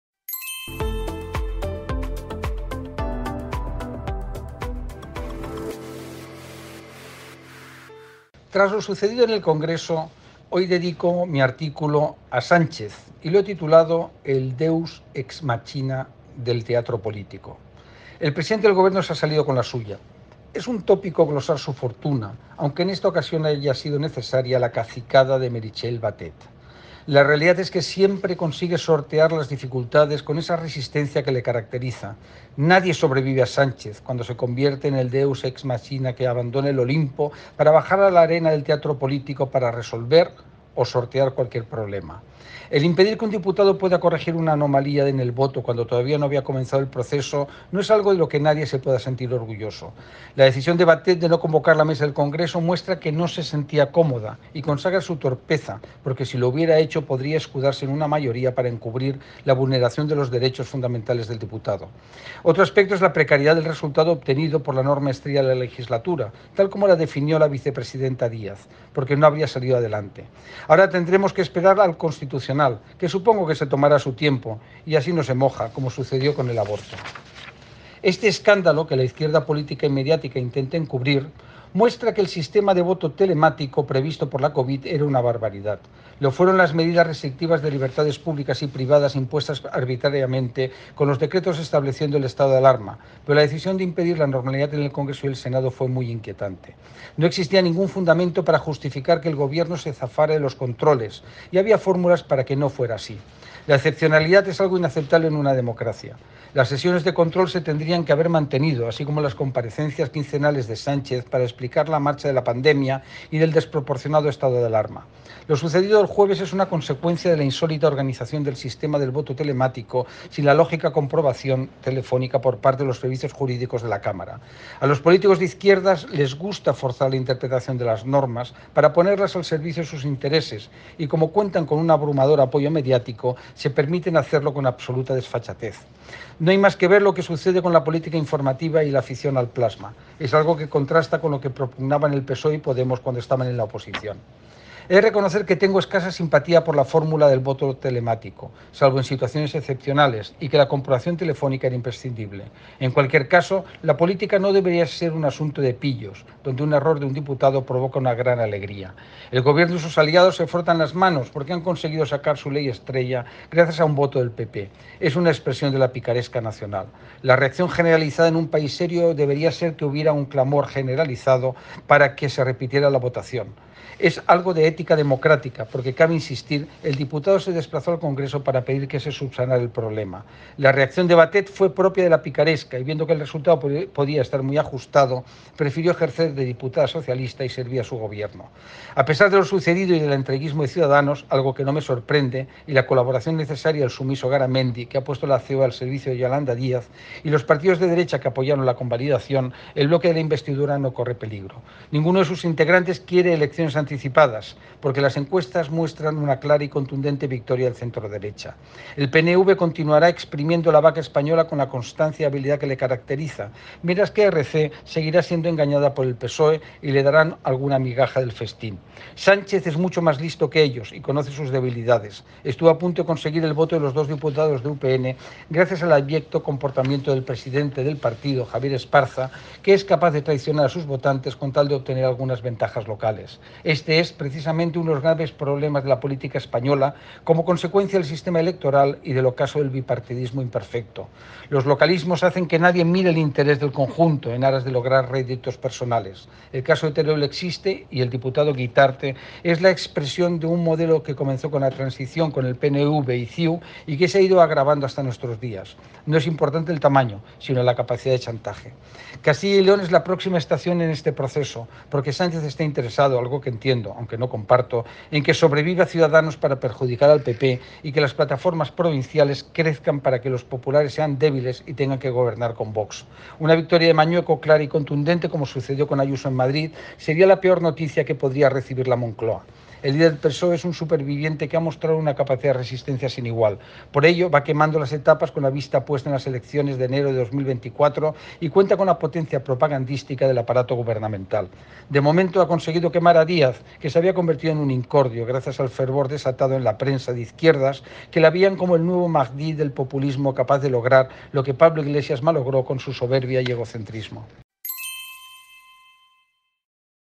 El Deus Ex Machina del teatro político
El presidente del Gobierno se ha salido con la suya. Es un tópico glosar su fortuna, aunque en esta ocasión haya sido necesaria la cacicada de Meritxell Batet. La realidad es que siempre consigue sortear las dificultades con esa resistencia que le caracteriza. Nadie sobrevive a Sánchez cuando se convierte en el Deus Ex Machina que abandona el Olimpo para bajar a la arena del teatro político para resolver o sortear cualquier problema.